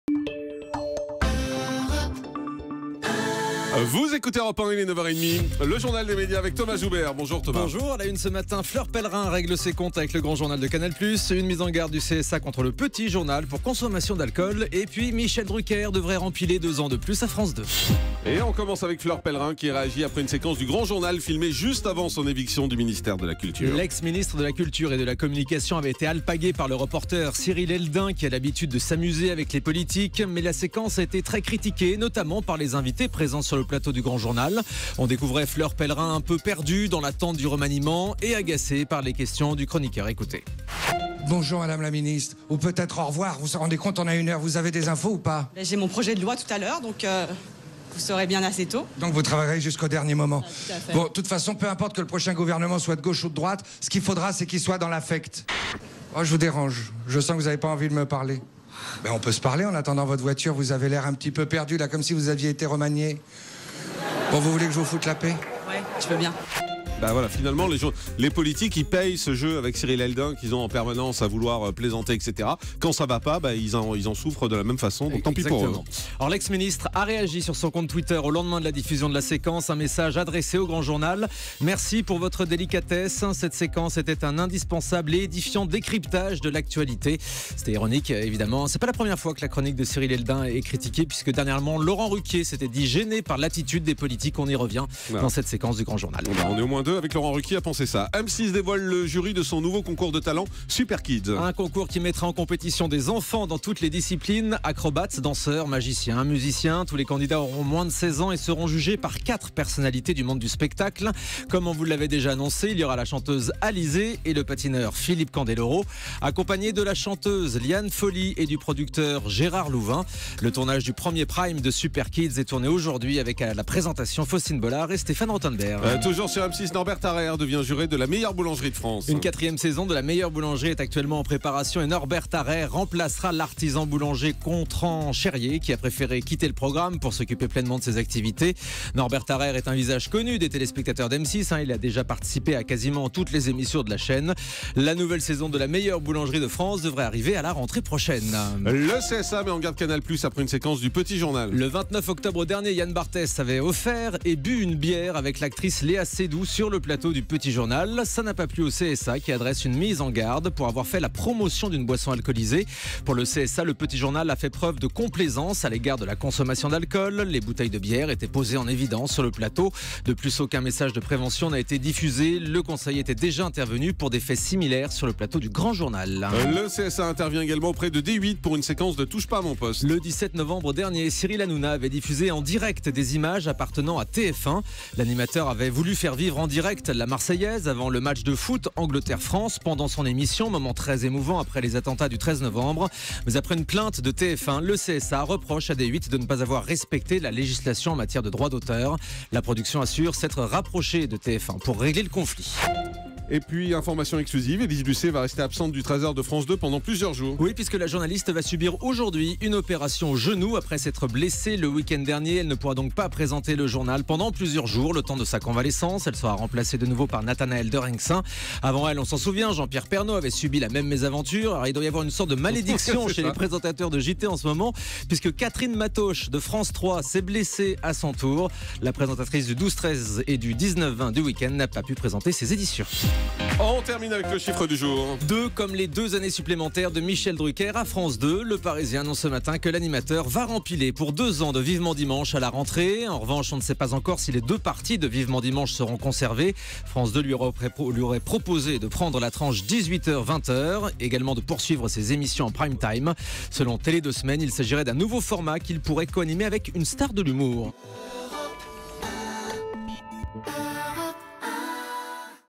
France 2 : deux nouvelles années minimum pour Michel Drucker
Selon Le Parisien, Michel Drucker a bien signé pour deux ans supplémentaires sur France 2 malgré la polémique sur les animateurs jugés trop vieux.